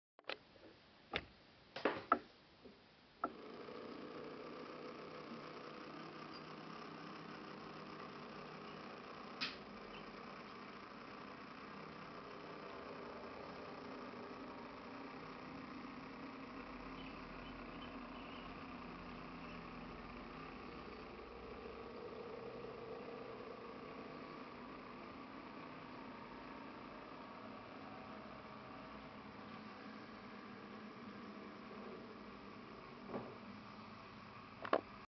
It's a video sketch - trying angles, light, depth of field and focus.  Engineer's day on Trevean Siding